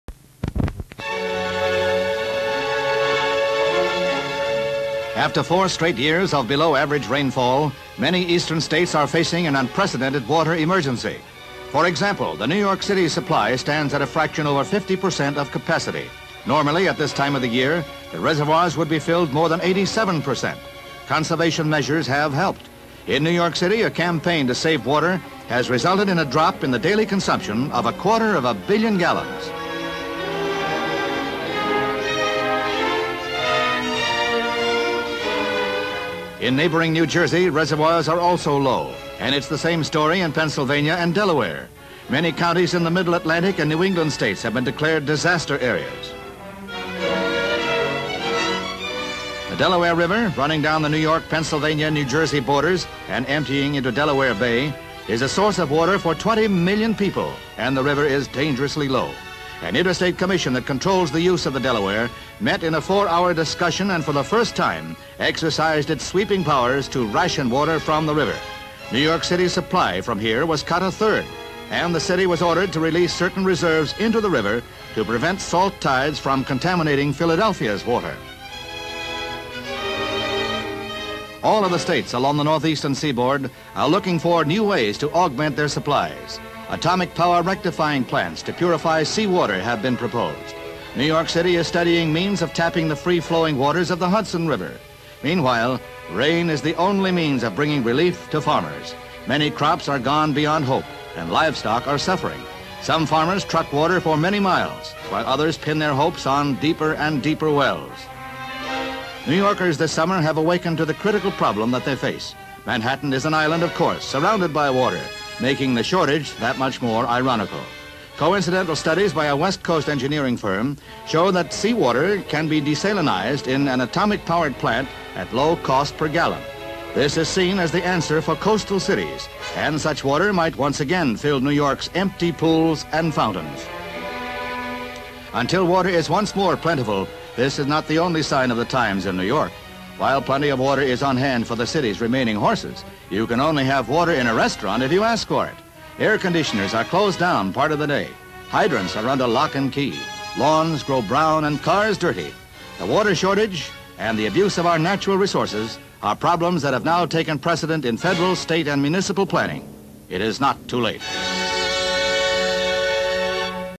The Water Crisis - H1700-31 | Footage Farm
13:09:16  LS of reservoir w/ low water level; MCU of barren bank of New York City reservoir.  Various shots of dried mud of bottom; rowboat chained to stump far from water.
13:09:49  Pan over New Jersey reservoir.
13:10:02  Shots of very shallow Delaware River.
13:10:33  CU sign:  Wanaque Reservoir - Help Keep The Water Pure.
13:10:34  More views of low levels of reservoirs.  Shots of sun thru trees.
13:10:49  CU man’s hand crumbling dirt.  Cattle in dry water-hole.  CU drilling deeper well.
13:10:59  LS from Empire State Building towards Central Park & views of Hudson & East Rivers.  Views of Manhattan skyline, Brooklyn Bridge; people sunbathing on pier.
13:11:18  Washington Square Park w/ empty fountain & arch behind; other dry fountains.
13:11:31  PIck-up truck w/ barrel & sign:  A.S.P.C.A. Free Water For Horses & man filling bucket; horse drinking.
13:11:41  CU fire hydrant locked.
13:11:46  Empty reservoirs w/ dried mud.